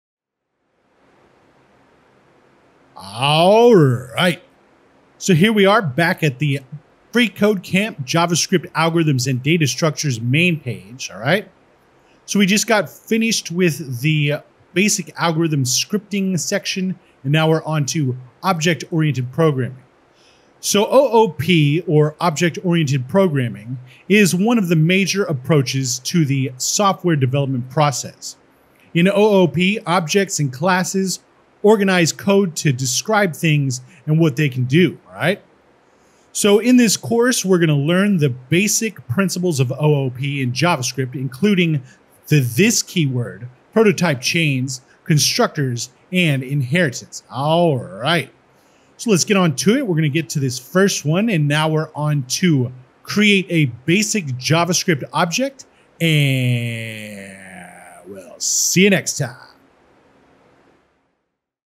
JavaScript Object Oriented Programming: Introduction | FreeCodeCamp
✨ **"Soulful Syntax: The Essence of Object-Oriented Programming"** ✨

Journey with us into the harmonious realm of Object-Oriented Programming (OOP). Just as nature is woven with intricate relationships and delicate balances, so is the fabric of our software, meticulously crafted with objects and classes.

🌲 **Awakening the Code**:

1. **Objects & Echoes of Reality**: Understand how 'objects' encapsulate data and actions, resembling living entities in our world, each with a story and purpose.
2. **Classes - The Blueprint of Dreams**: Delve into 'classes', the very soul blueprints that mold these entities, giving birth to their existence in our code.
3. **Whispers of 'this'**: Dance with the dynamic 'this' keyword, an ever-shifting reflection of the object it refers to.
4. **Prototypes & Eternal Chains**: Explore the ancestral lineage with 'prototype chains', the inheritance paths that echo the wisdom of the past.
5. **Constructors - The Breath of Life**: Witness the wonder of 'constructors', bringing entities to life, initializing them with purpose.
6. **Inheritance - Legacy of the Code**: Grasp the beauty of inheritance, where the lessons and traits of one are passed down, ensuring continuity and evolution.

🍃 **Harmony in Design**:

Object-Oriented Programming isn't just a coding paradigm; it's a philosophy, an art. In OOP, we see the interconnectedness of the universe reflected. Just as forests, rivers, and meadows interact in a delicate dance, so do the objects and classes in our code. This course illuminates not only the technicalities of OOP in JavaScript but also its soulful essence, its rhythmic dance.

Embark on this transformative voyage. Rediscover coding not just as a skill, but as a symphony of logic and creativity, of structure and fluidity.